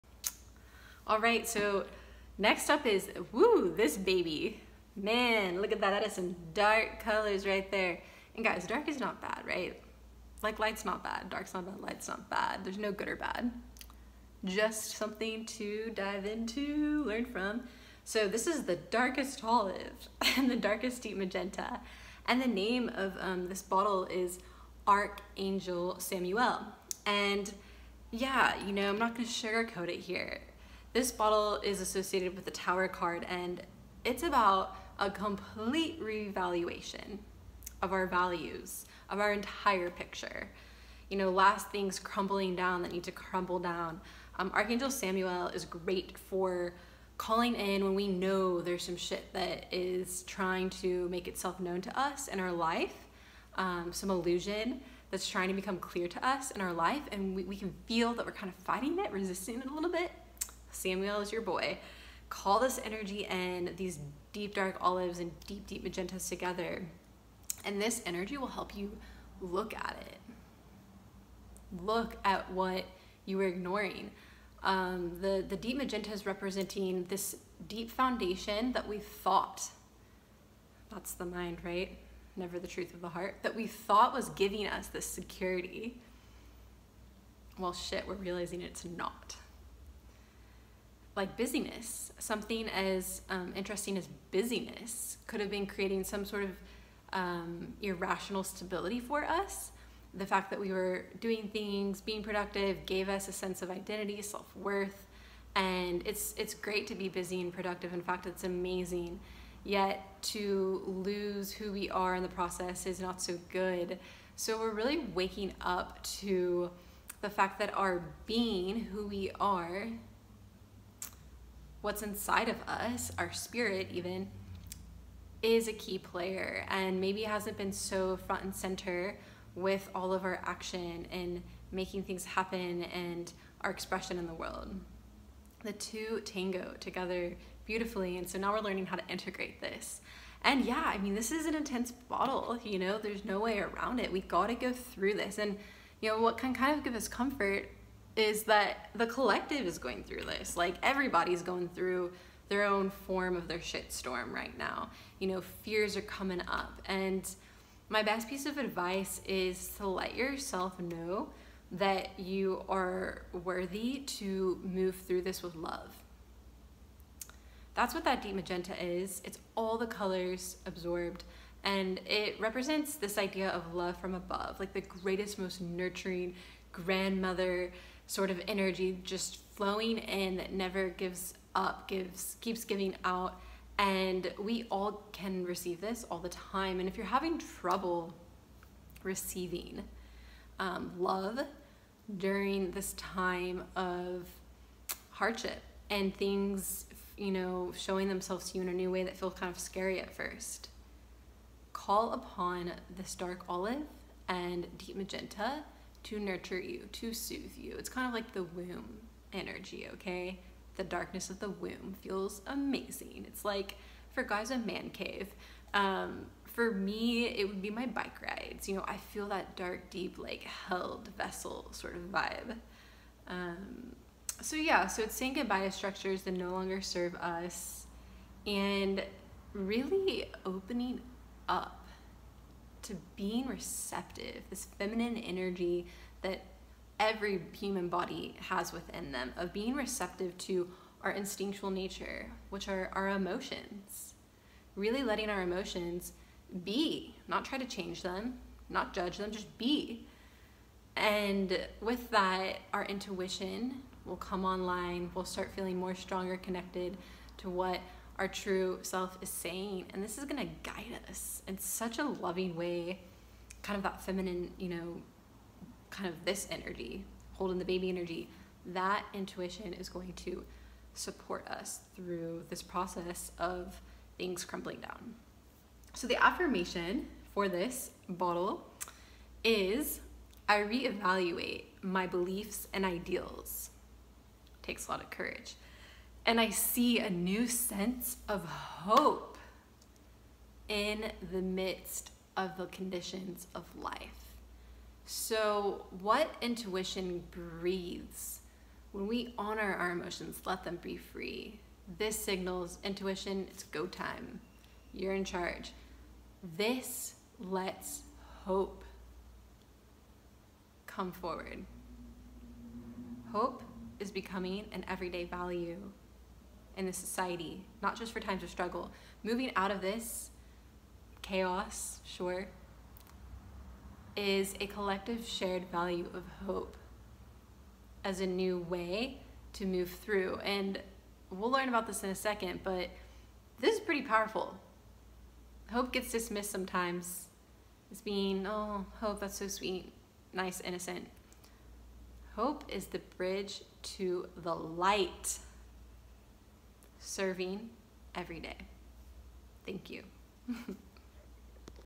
Archangel Samuel; Re-evaluation of the Big Picture
Archangel Samuel 102
Deep olive green/deep magenta
Foundations crumble, re-evaluation of everything:, life is in question, what it means to be alive. New hope deepened intuition to move through .
 Hope as a new everyday value.
◦ Rescue of hope in face of difficulty; re-evaluation of beliefs and values
◦ This angel supports us to face the truth of what we may wish not to see or turn away from
◦ Helps us to see where we may have built false sense of security.
That could be actually diluting our vitality and zest for life
✓ Structures that see now no longer serve us.
◦ Note on Patriarchy ; it’s not about bashing the patriarchy all this is is a light to remember the power of being feminine of being receptive to the higher guidance within of intuition ;
◦ this is divine feline leadership of the heart
◦ Tower tarot card ; complete re-evaluation of circumstances of life
◦ Affirmation: I re-evaluate my beliefs and ideals and  see a new sense of hope in the midst of the conditions of life.

◦ Gift ; o-live; energy to live that we may release the past of bitterness, forgive and experience freedom in a new sense of what it means to be alive ; a love of life itself